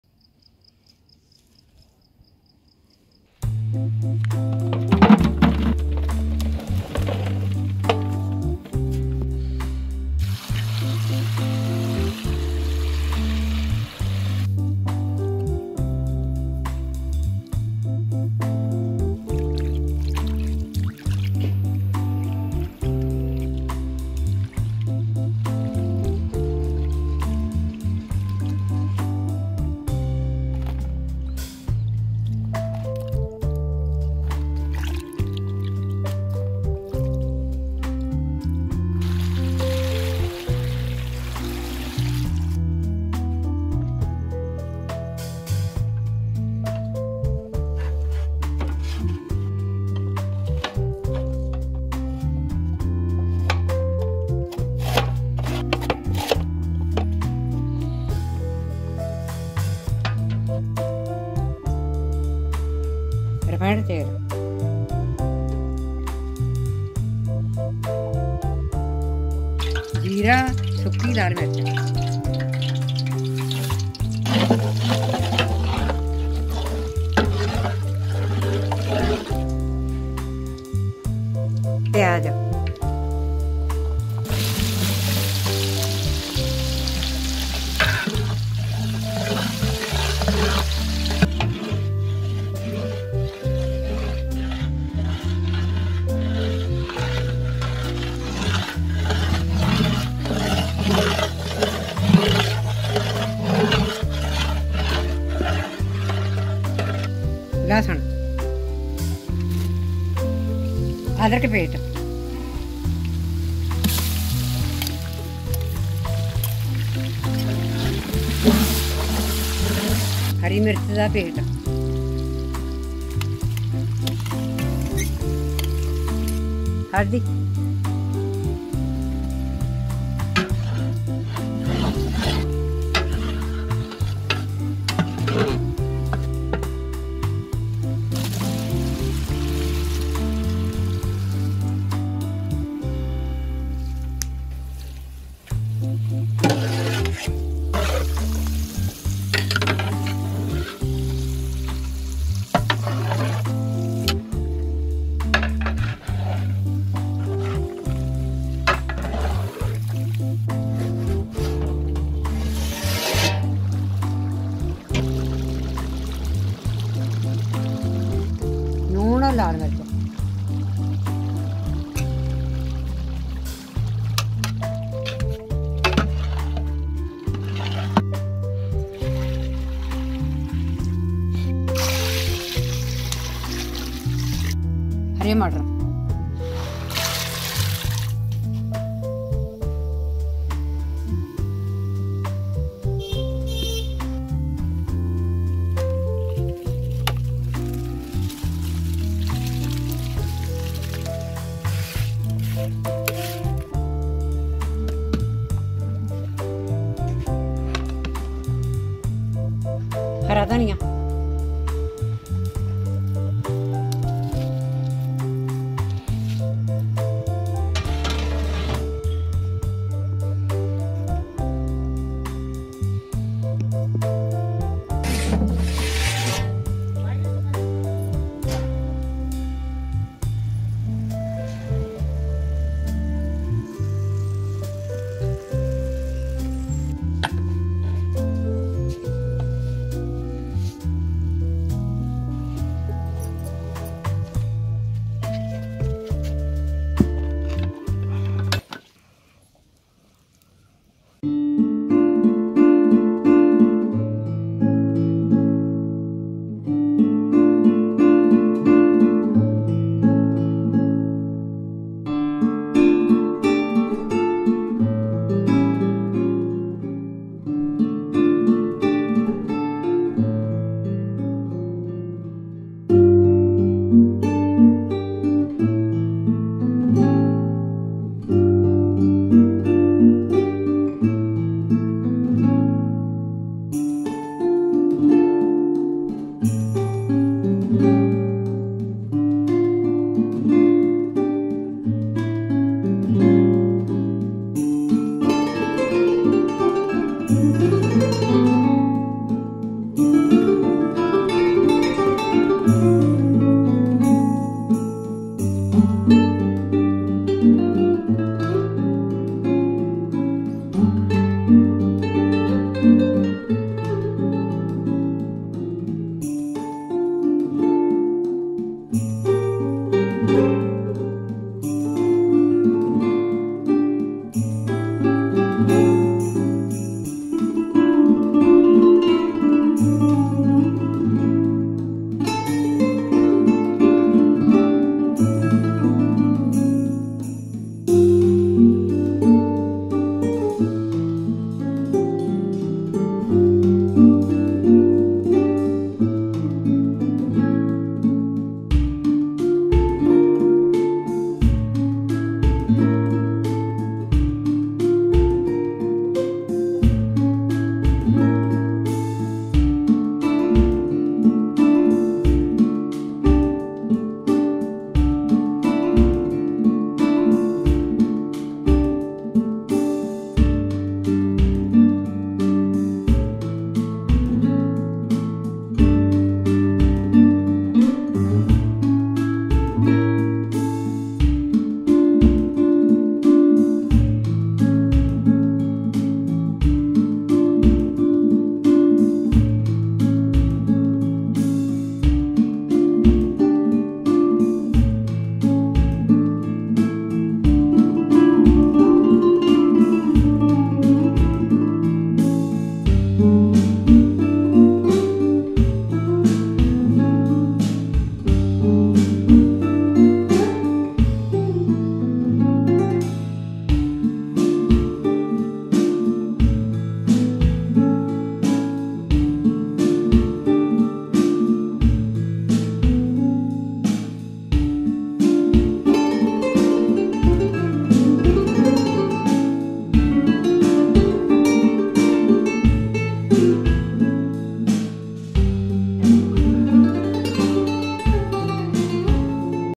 SIMPLE AND EASY TOMATO RICE RECIPE PREPARED BY MY GRANNY | THAKKALI SADAM - TOMATO PULAO/PULAV
Watch this video on how to prepare Tomato Rice. Here is an easy and simple way of doing it and this tastes really good.
there is lots of recipes which can prepared with tomatoes like
tomato soup , tomato curry, tomato recipe, tomato chutni ,today we prepared tomato rice recipe
with our village style method
hope u like this reccipe